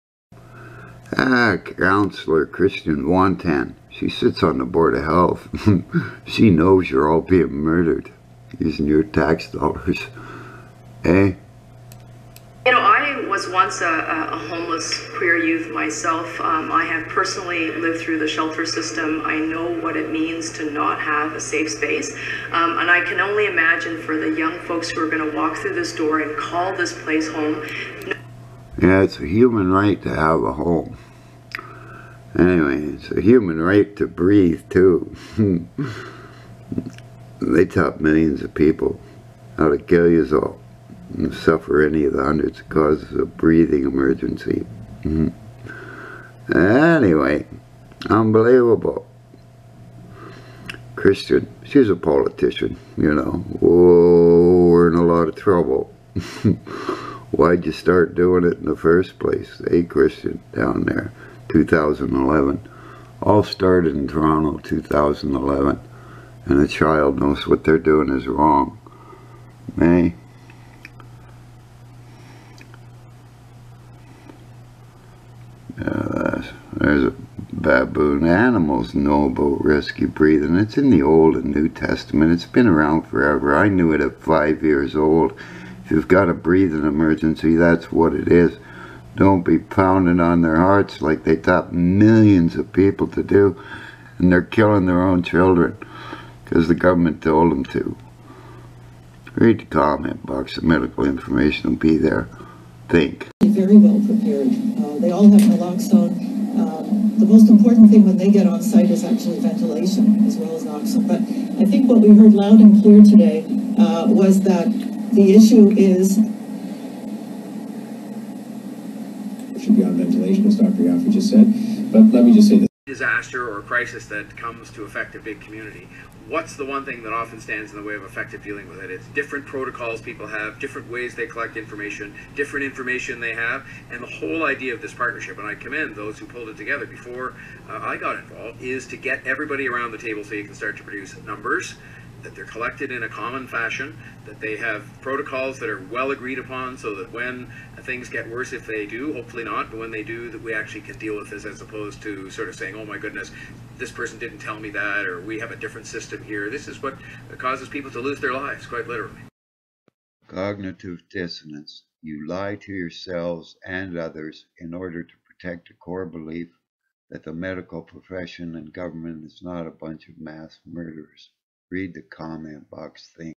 Councillor Kristyn Wong-Tam The Good The Bad and The Downright Ugly Government etc USA and Canada
Lying Trump says he had no breathing problem COVID His Doctor said he was given oxygen

My phone call White House Secret Service agents have First Aid know millions have been taught murder Comment boxes medical info

State sponsored psychological terrorism workplace harassment/bullying needless deaths stress increasing mental/physical illness drug use/misuse. Done on purpose
Be safe. Be kind. First, do no harm @GaryCPR                                                                                Original post Kristyn Wong-Tam